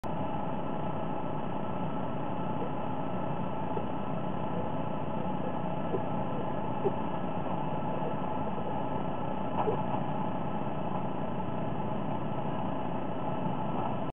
2011 Topeka Falcon Cam-March 15, 2011 @ 1127 hrs EST.
2011 Topeka Falcon Cam-March 15, 2011 @ 1127 hrs EST. It really does look like Hope is kinda miserable, don't you think?